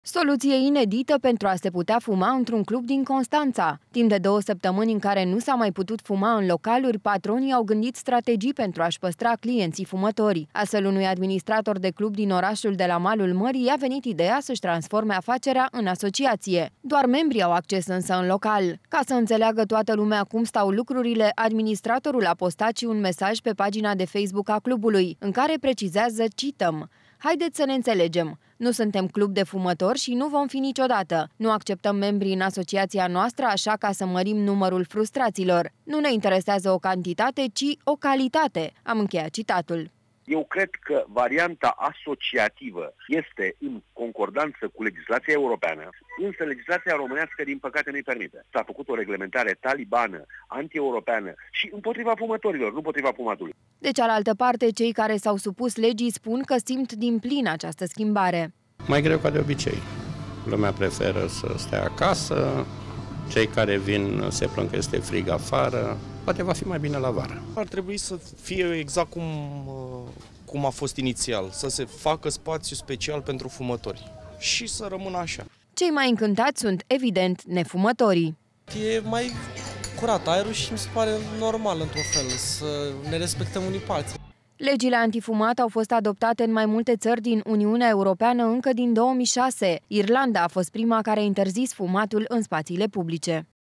Din cauza legii antifumat, administratorul unui club își transformă afacerea în asociație
Soluție inedită pentru a se putea fuma într-un club din Constanța! Timp de două săptămâni în care nu s-a mai putut fuma în localuri, patronii au gândit strategii pentru a-și păstra clienții fumători. Astfel, unui administrator de club din orașul de la malul mării i-a venit ideea să-și transforme afacerea în asociație. Doar membrii au acces, însă, în local.